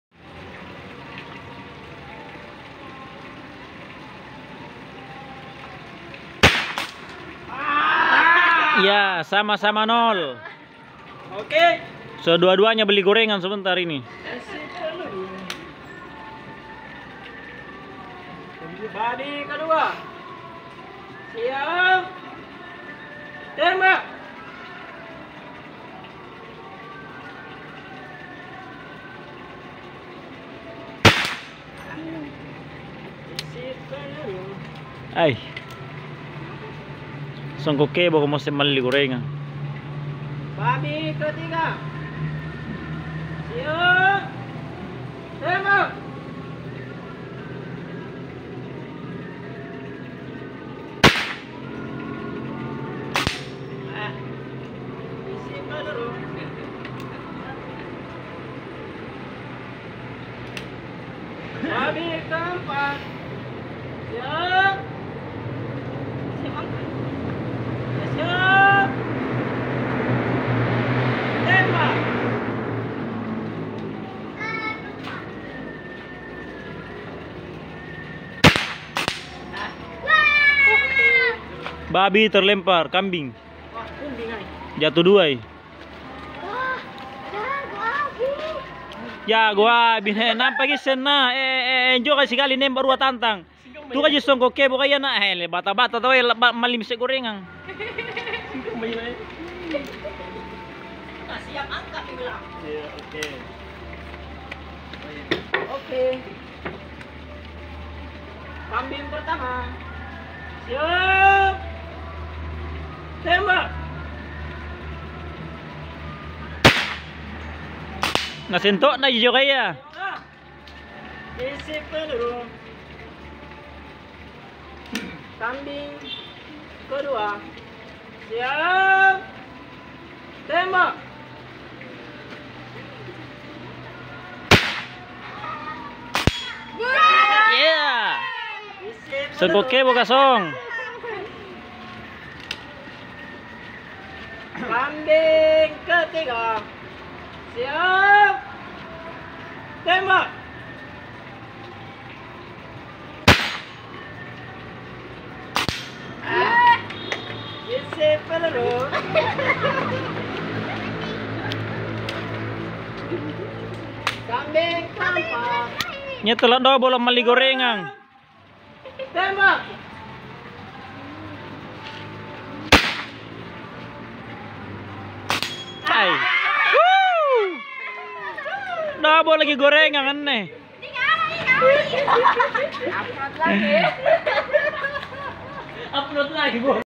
LATIHAN MENEMBAK METAL!!BERHADIAH SARABBA DAN GORENGAN (tantangan dr om awi hari kedua)
kegiatan ini berlangsung tanggal 14 januari 2021, lanjutan dari tantangan sebelumnya yang om awi' tidak puas karena kalah, ternyata. "MASIH KALAH JUGAAAA"